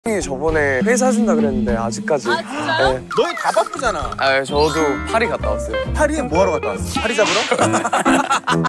Joshua was laughing through the pain 🤣
Source: Harvesting in a 6600m2 rice field with "VERY NICE" friends